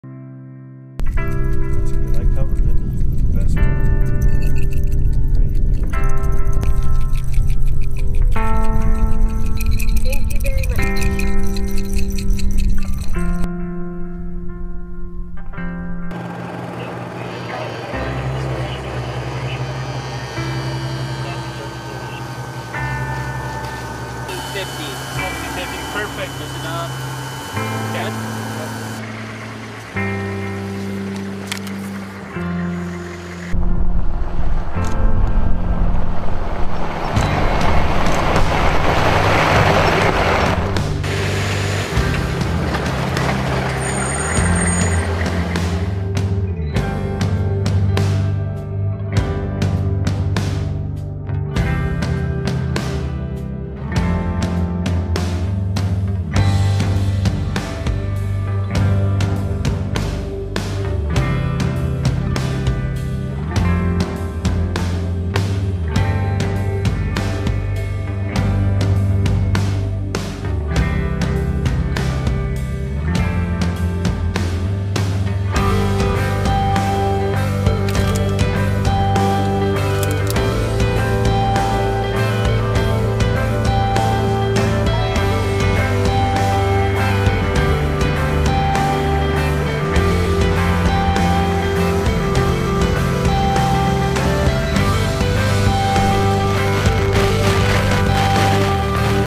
Chemical and Biological Warfare Cross-Contamination Study at Dugway Proving Ground
A light vehicle cross-contamination study being conducted at Dugway Proving Ground assess how chemical and biological warfare agent is transferred to vehicles from contaminated personnel and terrain.
(Video produced by Gabriel Archer, Dugway Visual Information Specialist)